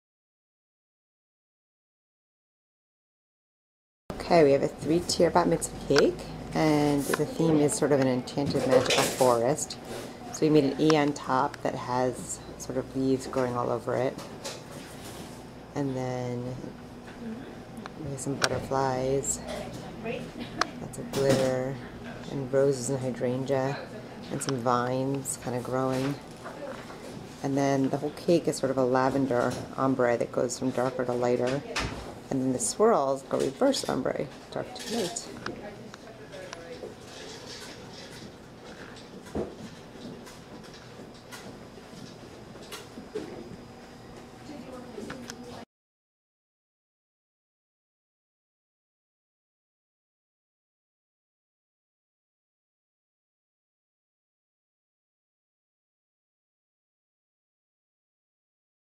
Lavender Ombre Bat Mitzvah cake
We partnered with our client and Phyllis Skolnick from Extraordinary Productions to design this Bat Mitzvah cake. It includes an ombre color fade from light lavender at the top of the cake to a darker lavender at the base. The glittery swirls decorating each tier include a contrasting ombre pattern in the opposite direction.